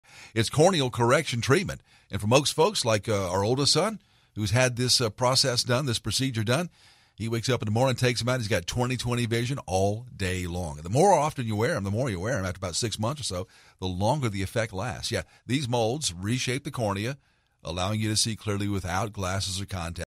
Gary David Son Did CCT
Corneal Correction Treatment Freedom from Glasses & Contacts NO Surgery Gary David from WVOC 103.5 Son Use to wear glasses & contacts No he sees 20/20 Vision Without them in One Night